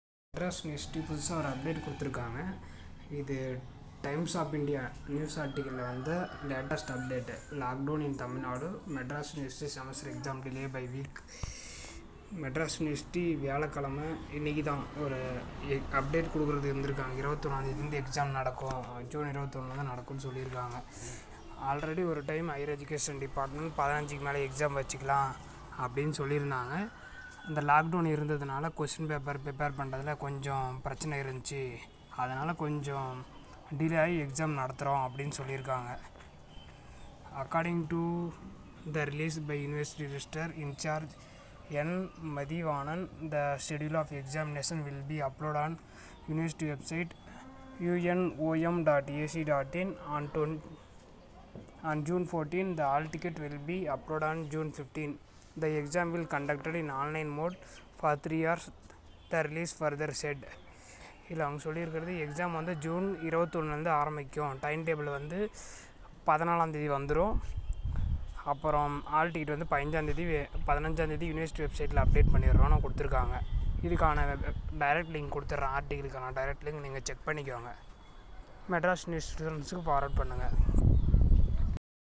Madrasuniversity Chennai Notification Examrelated onlineexam
Timetable will release on June 14

Hall ticket will update on June 15

Exam begin from June 21 mentioned in article

Direct link for article

University department admission going on for
Madras University.
Periyar University
Madurai kamaraj University
Alagappa University
Bharathidasan University

OTHER UNIVERSITIES ADMISSION ALSO GOING ON